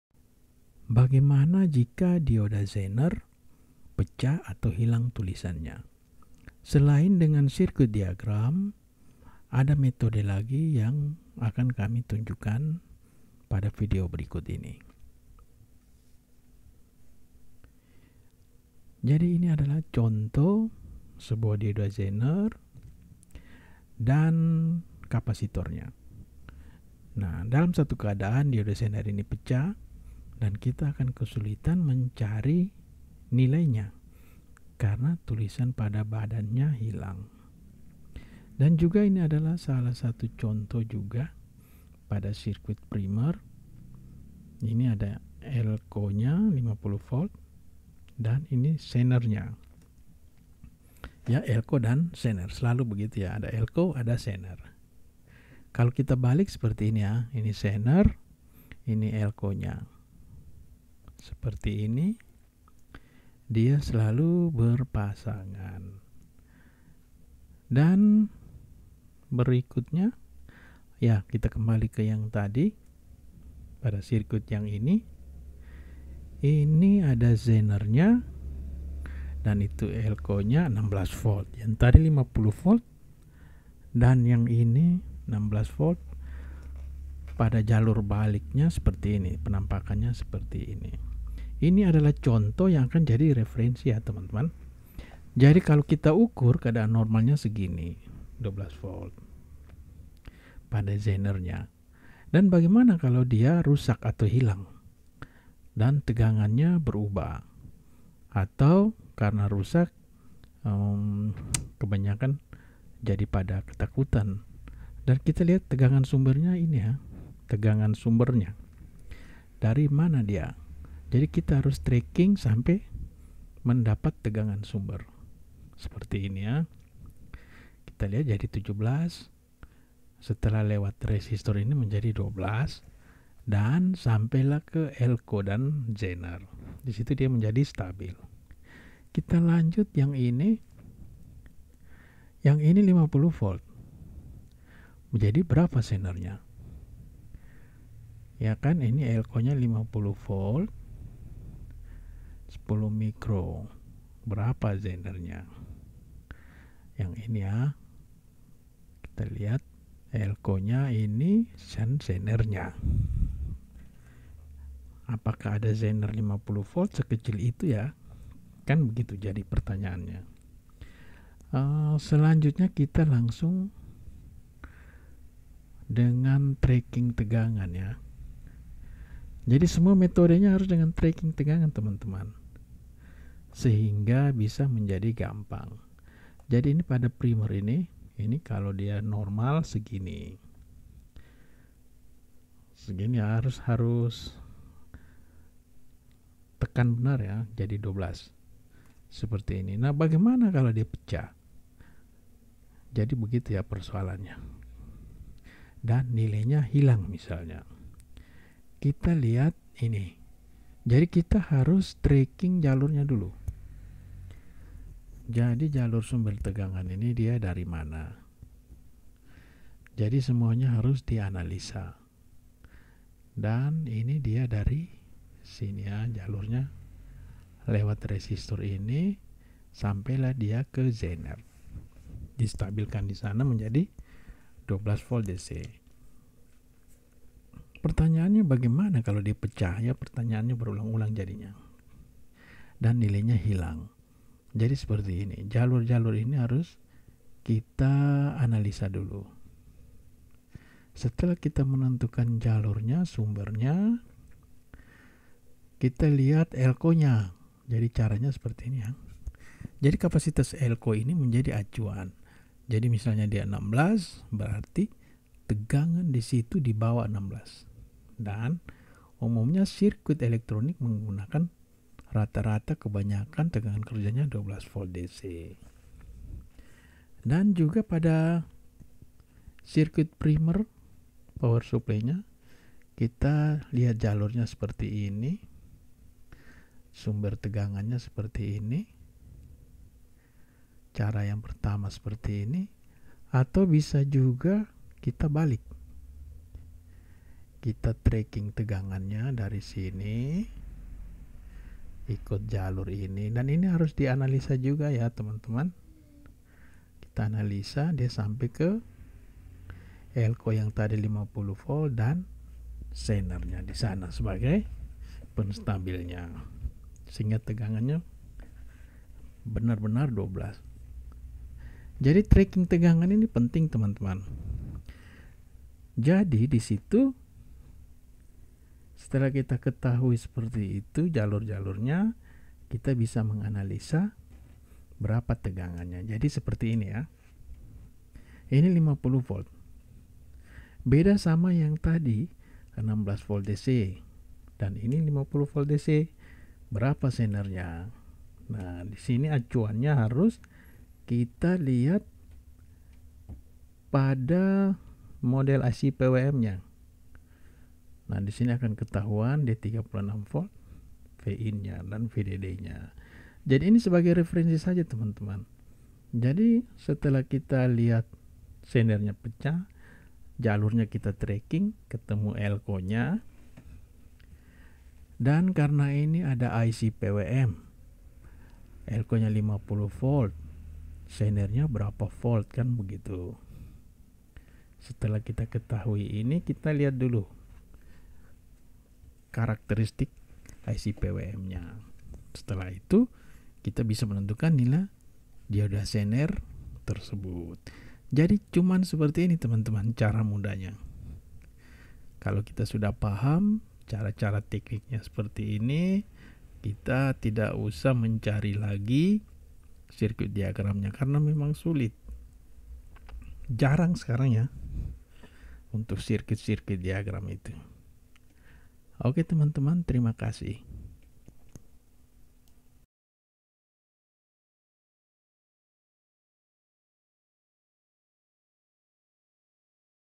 CARA MENGETAHUI NILAI DIODA ZENER YANG HILANG TULISANNYA ATAU PECAH
VIDEO CARA MENCARI TAU NILAI DIODA ZENER YANG HILANG TULISANNYA ATAU PECAH SEHINGGA SULIT DALAM MEREPARASI